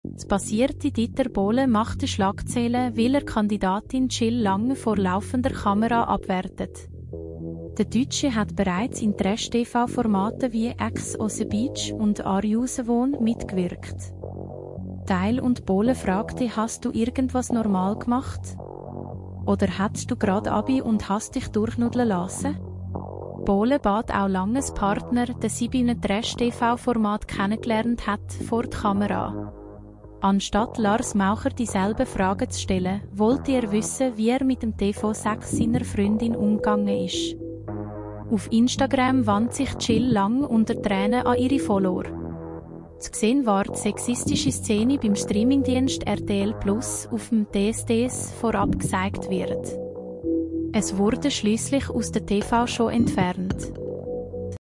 DSDS: Jill Lange platzt nach Ankündigung von neuer Staffel der Kragen
Das passierte  Dieter Bohlen machte Schlagzeilen, weil er Kandidatin Jill Lange vor laufender Kamera abwertete. Der Deutsche hat bereits in Trash-TV-Formaten wie „Ex on the Beach“ und „Are You The One?“ mitgewirkt. Teil und Bohlen fragte „Hast du irgendwas normal gemacht? Oder hattest du gerade Abi und hast dich durchnudeln lassen?" Bohlen bat auch Langes Partner, den sie bei einem Trash-TV-Format kennengelernt hatte, vor die Kamera. Anstatt Lars Maucher dieselben Fragen zu stellen, wollte er wissen, wie er mit dem TV-Sex seiner Freundin umgegangen ist. Auf Instagram wandte sich Jill Lange unter Tränen an ihre Follower. Zu sehen war die sexistische Szene beim Streamingdienst RTL+, auf dem „DSDS“ vorab gezeigt wird. Es wurde schließlich aus der TV-Show entfernt.